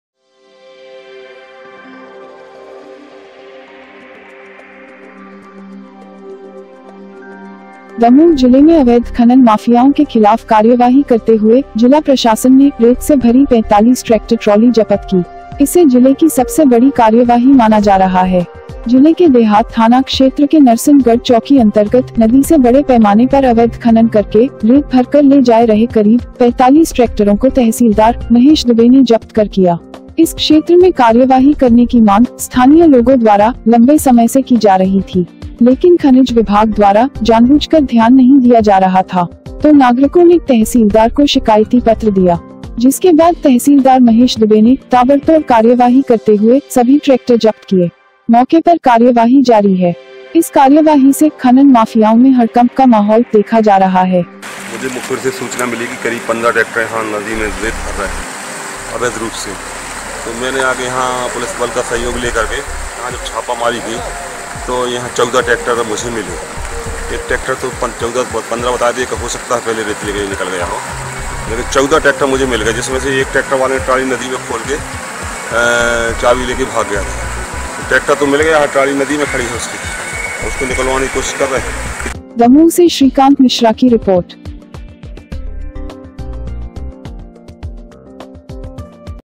रेत से भरी पैंतालीस ट्रेक्टर ट्रॉली जप्त की - MP NEWS NETWORK DAMOH
दमोह जिले में अवैध खनन माफियाओं के खिलाफ कार्यवाही करते हुए जिला प्रशासन ने रेत से भरी पैंतालीस ट्रेक्टर ट्रॉली जपत की । इसे जिले की सबसे बड़ी कार्यवाही माना जा रहा है।